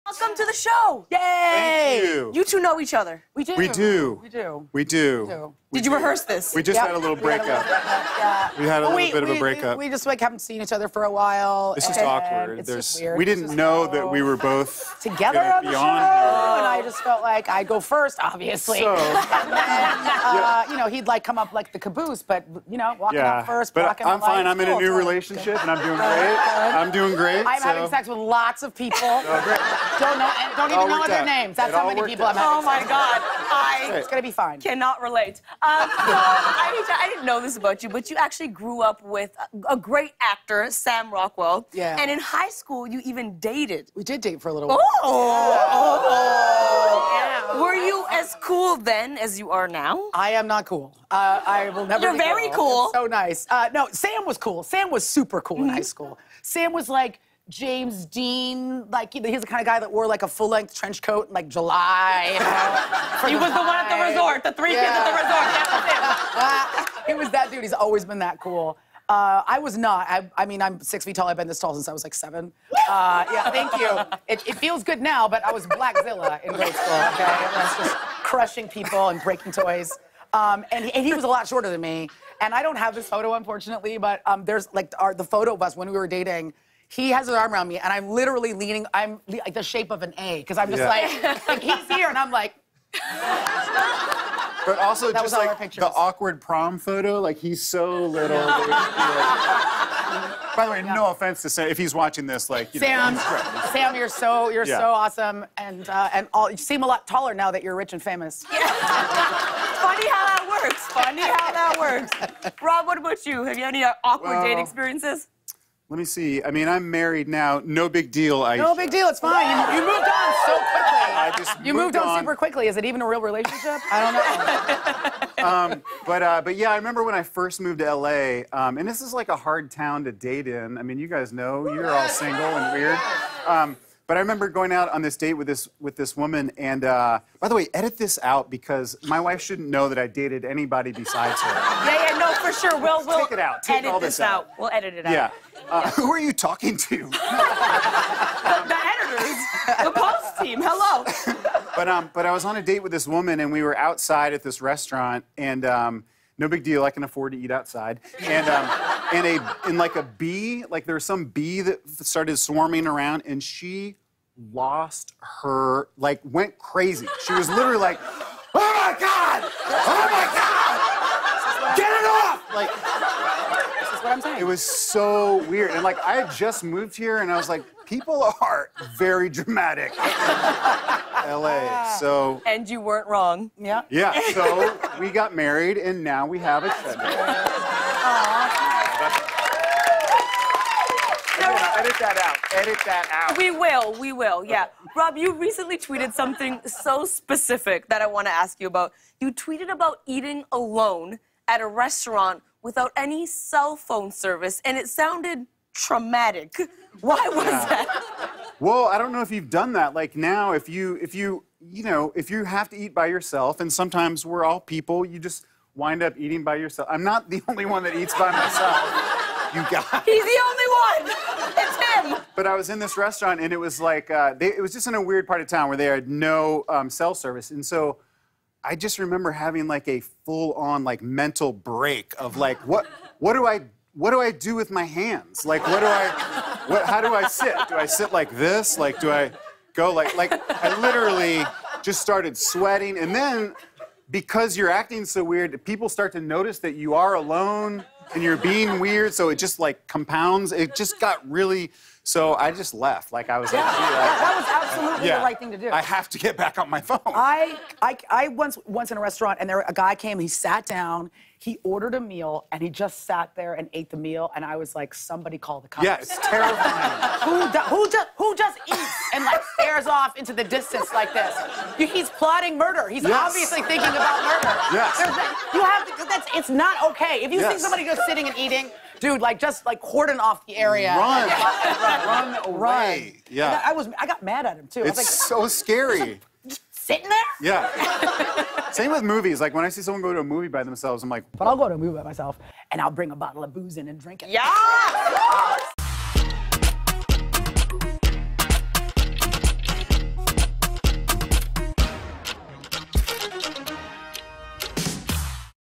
Aisha Tyler Dated Sam Rockwell in High School
Aisha Tyler discusses dating Sam Rockwell in high school and Rob Huebel shares a story about eating alone at a restaurant… with no cell phone service.